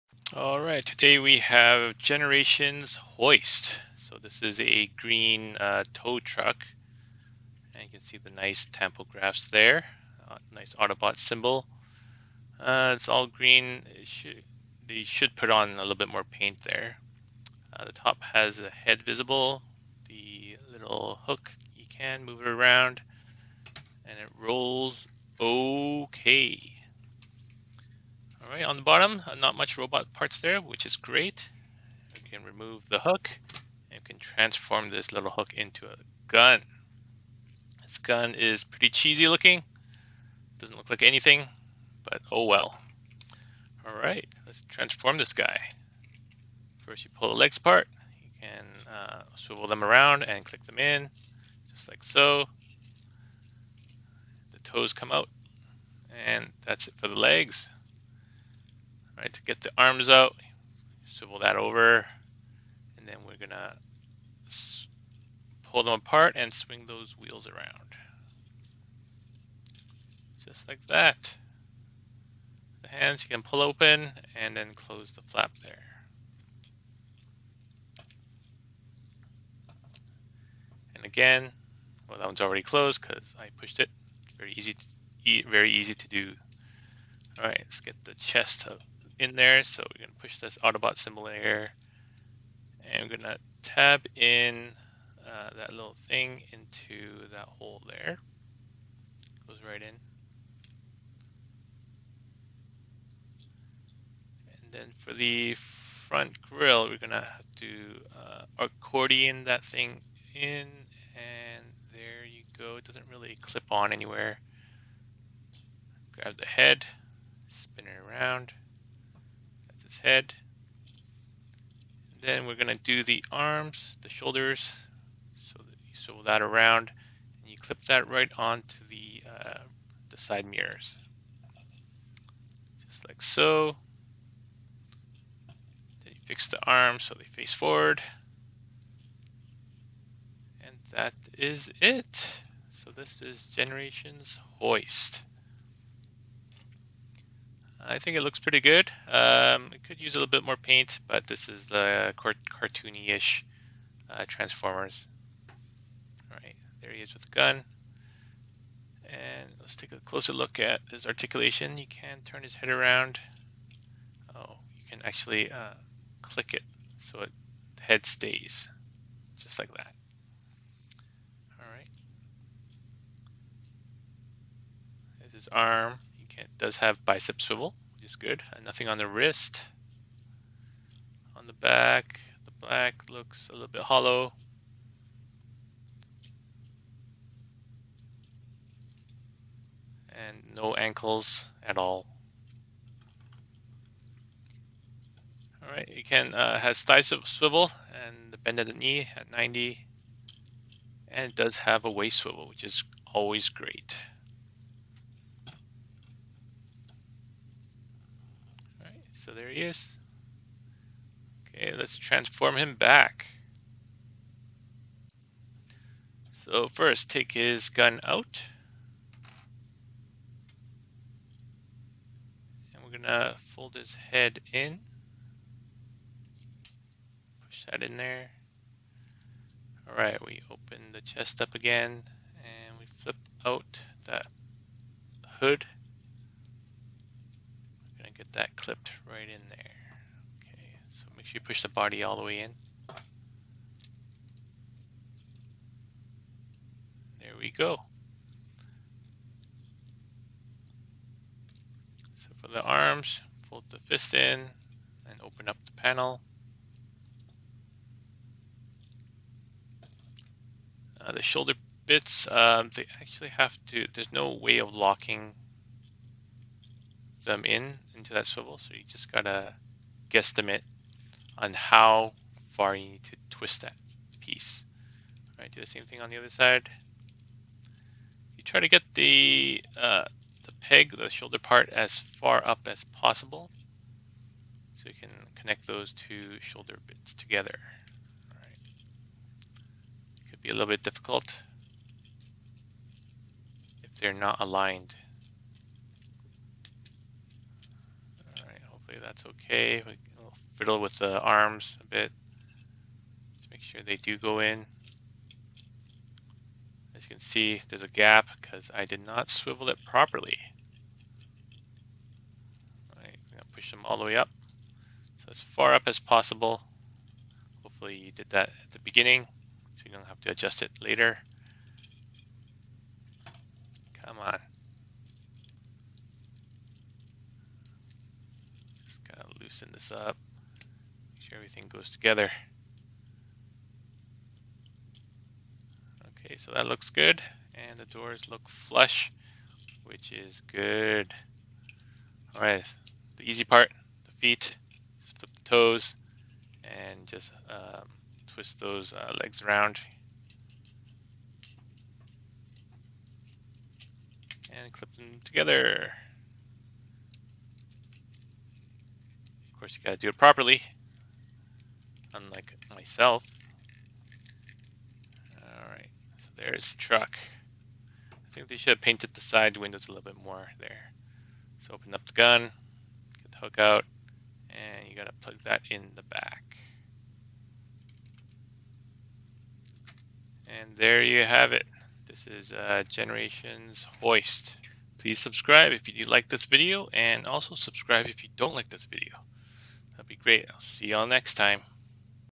Transformers Generations Deluxe Hoist Review
Here is a look back at some of my older Transformers.
Generations Deluxe Hoist from 2013.